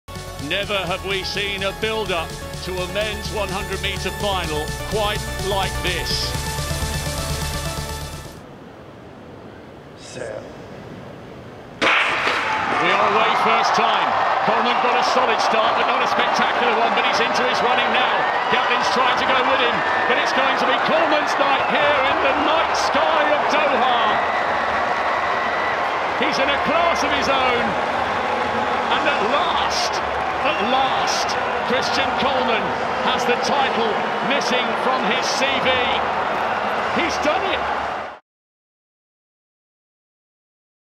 Coleman wins the 100m | World Athletics Championships 2019 | Doha Moments
Christian Coleman lit up the Khalifa International Stadium on Day 2 of the 2019 World Athletics Championships in Doha with a stunning 9.76s run to win gold in the men's 100m.